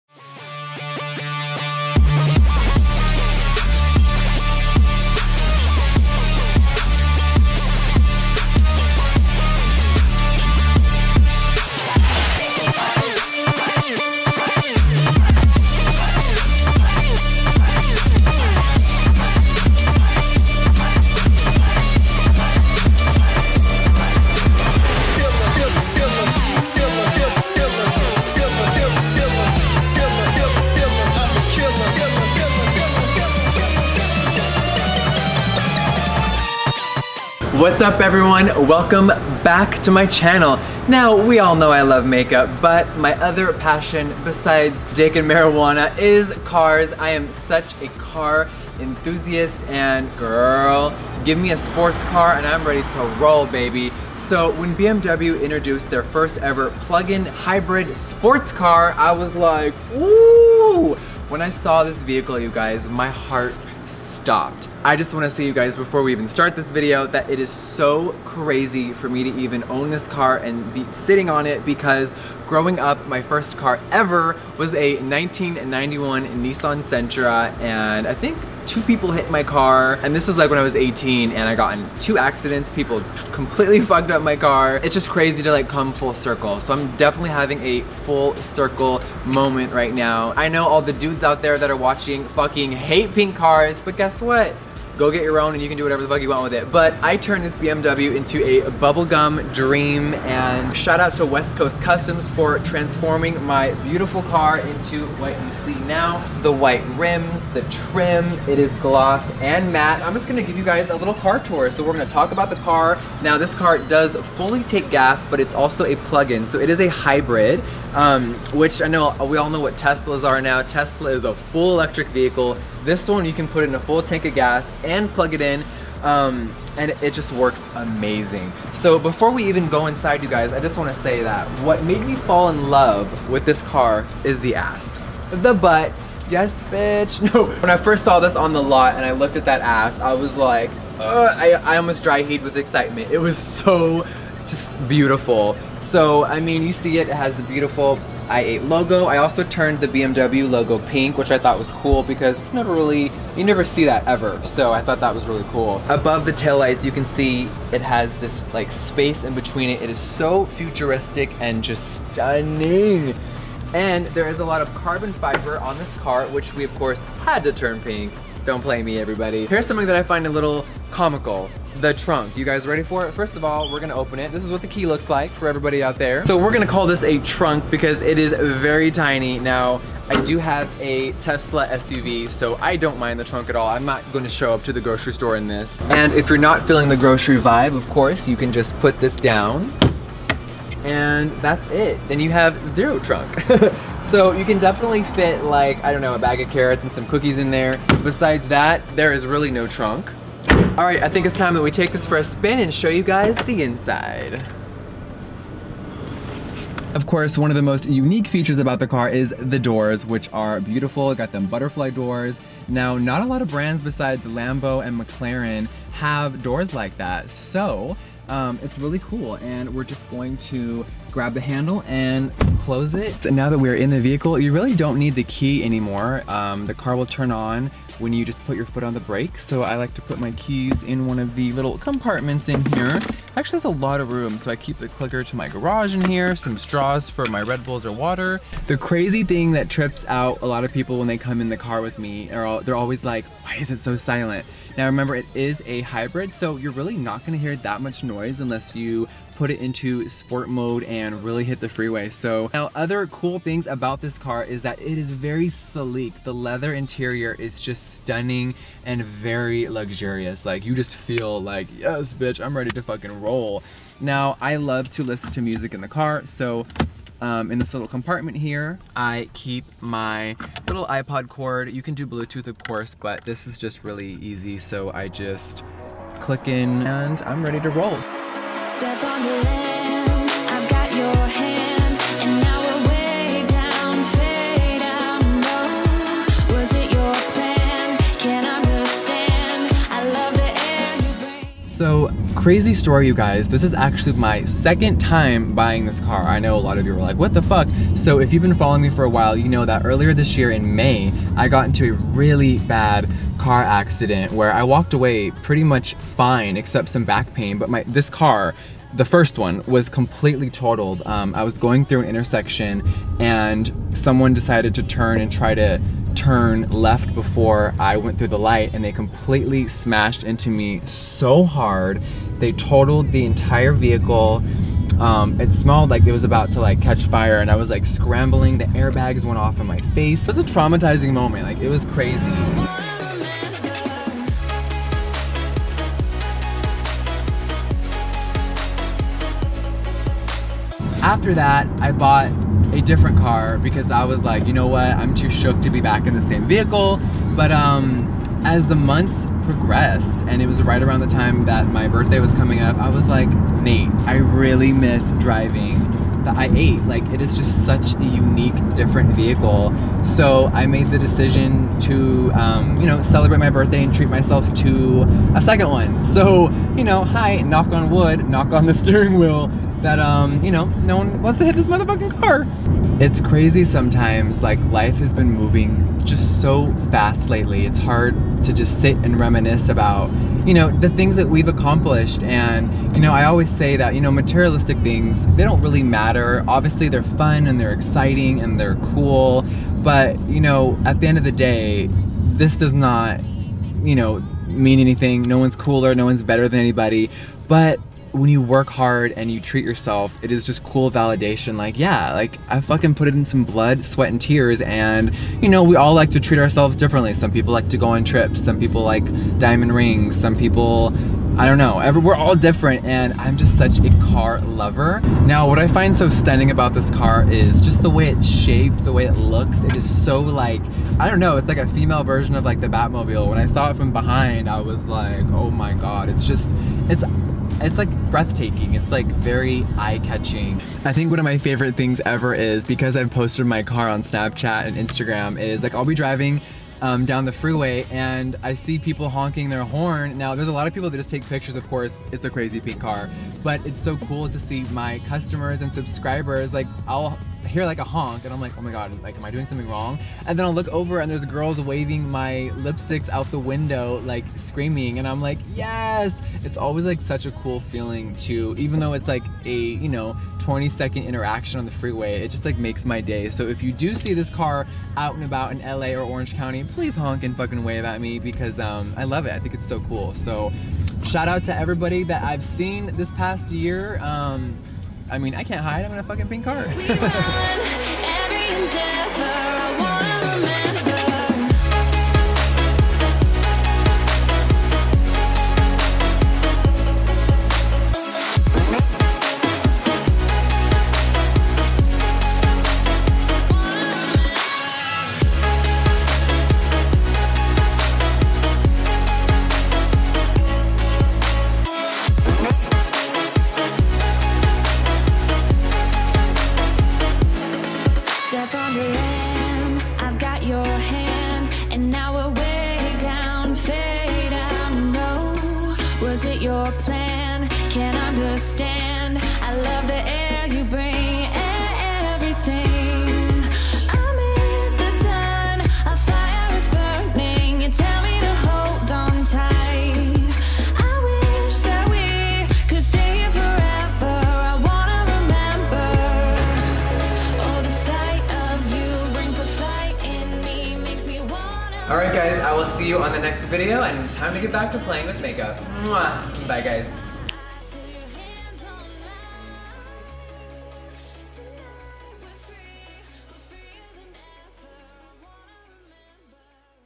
MY NEW BARBIE BMW i8 | Jeffree Star [4K Ultra HD]
THIS VIDEO CAN BE VIEWED IN 4K! [Ultra HD] - HEY EVERYONE! Today I'm putting down the makeup brushes and taking you to my home in Calabasas where I give a tour of my *NEW* Barbie custom pink BMW i8! The design was created by the wizards at West Coast Customs! (Thanks guy, you SLAYED this!!) They custom made this shade of pink for this car and I was so stunned when I saw it for the first time! Beside being obsessed with highlighters and lipsticks, I LOVE sports cars and have been a car fanatic since I was a kid!

For my birthday last month, I bought this beautiful BMW i8! In this video, I go for a drive and talk about some of my past... getting into a car accident and starting from nothing. My first car I ever had when I graduated high school, was a 1991 Nissan Sentra that 2 people hit and almost totaled... later on in life,  I would drive that beat up old car to freelance makeup jobs and hustle to make rent money and $ to eat... It's crazy that 10 years later my entire life has changed. I never gave up on my dreams and never let anyone tell me "NO" - Your dreams can be a reality... But the road to get there will not alway she pleasant or fun. I'd never give up all of my struggles and experiences. It's made me into the person I am today.

Thank you for making 2016 such an incredible journey and welcoming me on YouTube! It's been an amazing first year as a "YouTuber" as they say haha Can't wait to see what 17 brings us... LOVE YOU. I could never say that enough... XOXO

Video was shot and edited by: Christian Henry Smith
Drone footage by: Nathan Schwandt
Jacket by: YSL
Jeans by: Gucci
Shoes by: Christian Louboutin
Sunglasses by: Porsche Design